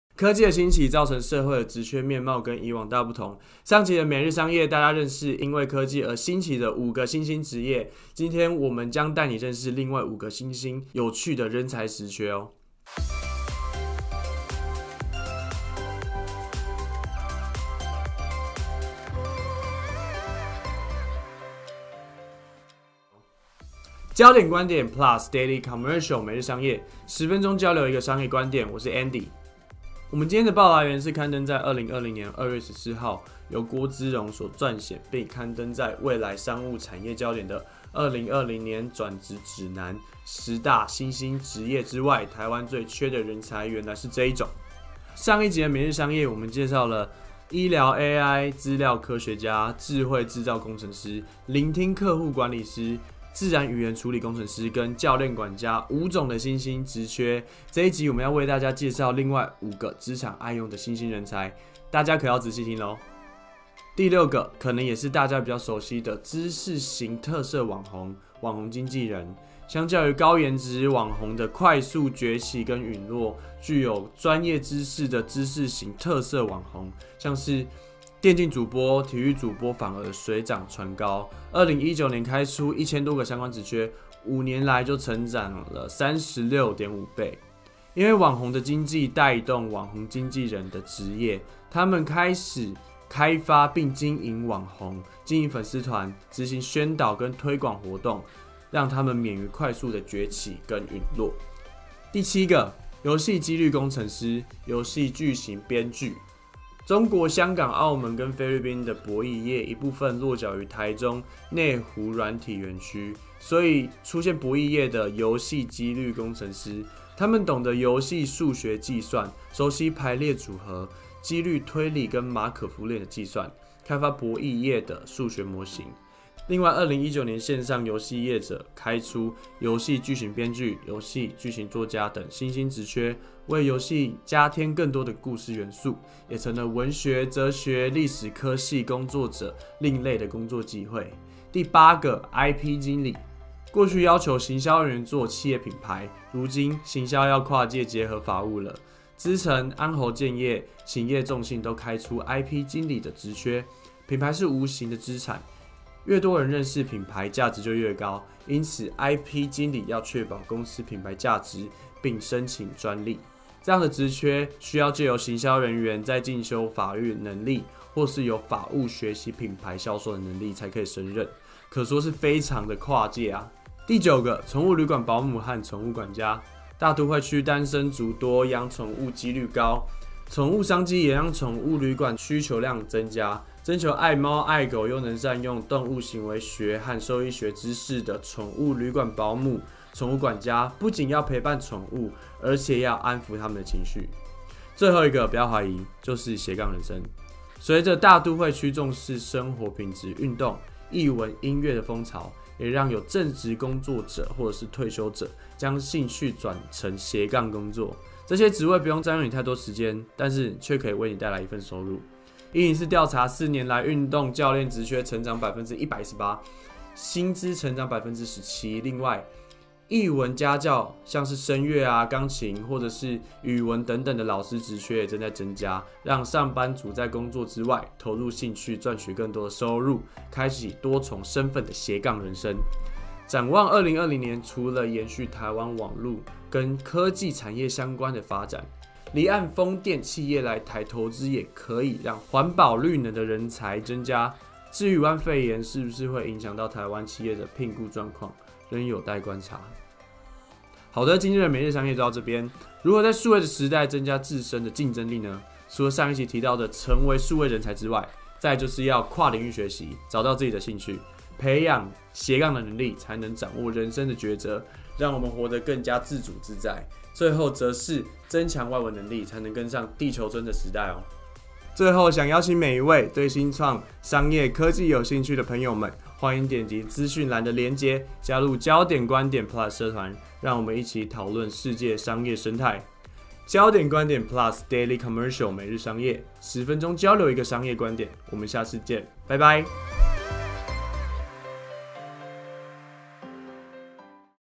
【每日商業EP25】2020轉職指南！10大新興職業之外，台灣最缺的人才原來是這一種（下）
今天每日商業的報導來源是2020年02月14號刊登在未來商務產業焦點的【2020轉職指南！10大新興職業之外，台灣最缺的人才原來是這一種】

在快速變遷的數位科技及AI時代，除了生技、醫療AI資料科學家以外，你有沒有聽過「自然語言處理工程師」及「聆聽客戶管理師」呢？網紅經濟帶動「網紅經紀人」職業，你還知道有哪些因時代而生的新興職業嗎？